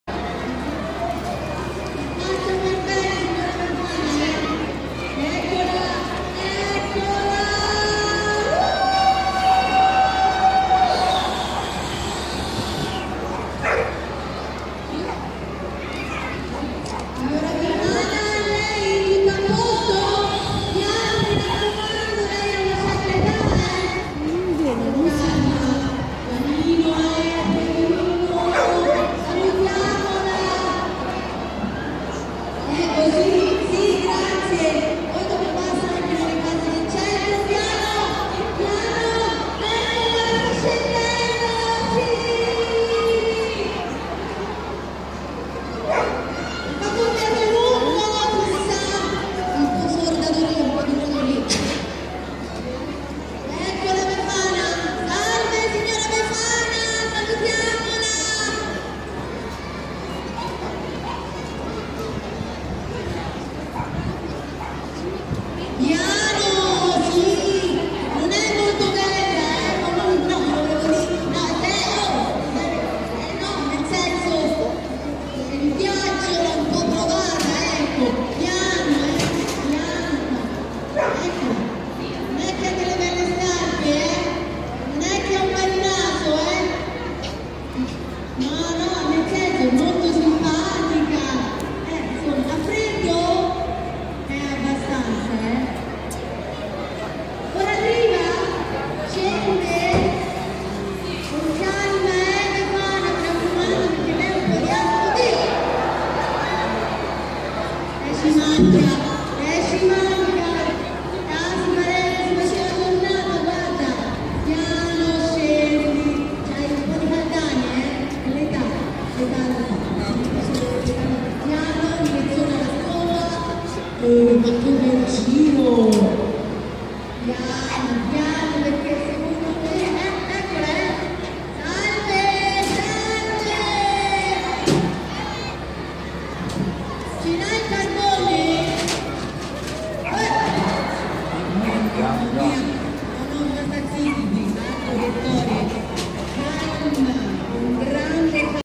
Grosseto - L'arrivo della Befana dal palazzo Aldobrandeschi - 05/01/ 2017
Sotto ad un foltissimo pubblico di bambini, nonostante il freddo pungente, è arrivata la Befana! Dalla torre più alta del palazzo Aldobrandeschi, sede della   Provincia di Grosseto, la Befana si è manifestata, annunciata dal classico fumo arancione.
I vigili del fuoco di Grosseto, ogni anno, regalano ai bambini un articolato ed animato spettacolo irrinunciabile anche dai grandi.
"Viva la Befana!"